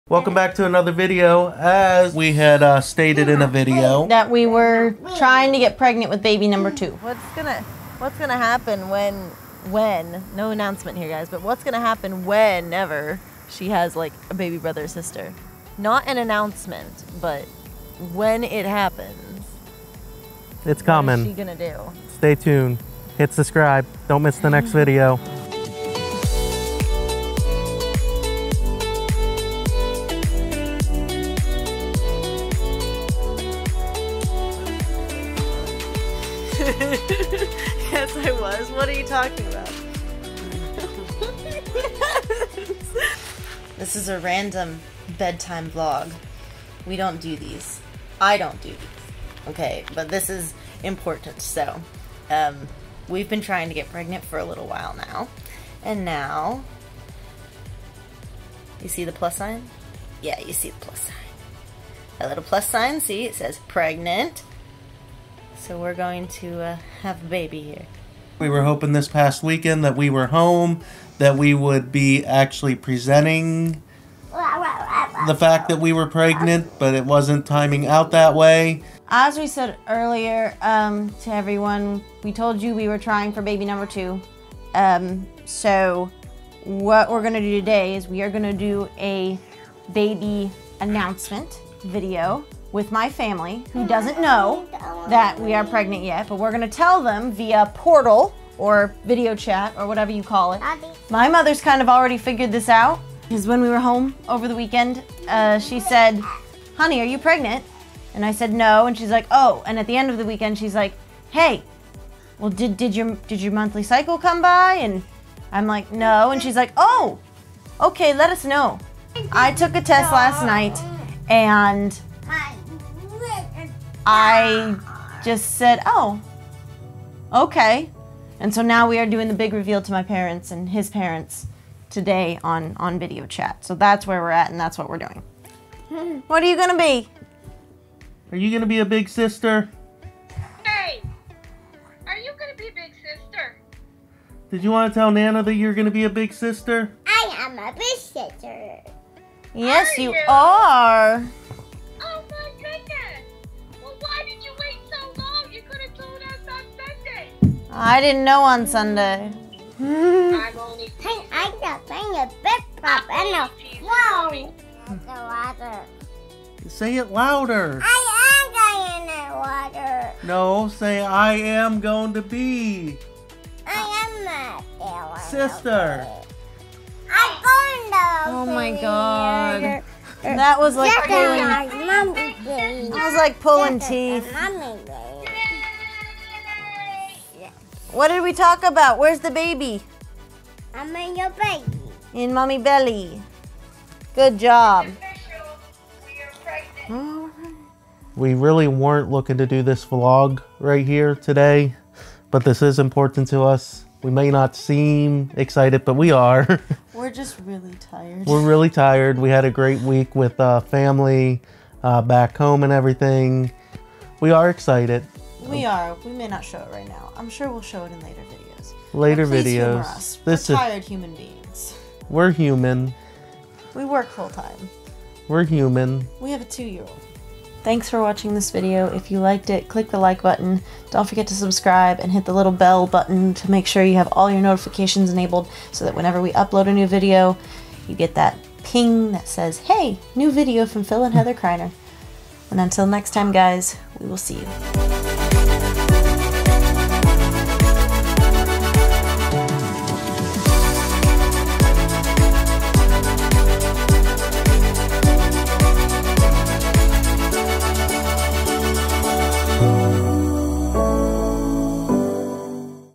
Pregnancy Announcement | We're Having A Baby
We've been trying to get pregnant for a while. This year caused a lot of unexpected stress, which takes a toll on the human body. But we finally made it (literally)!
And Serenity just couldn't seem to get out, "I'm going to be a big sister!" Ha ha! You'll have to forgive her; she is only two after all.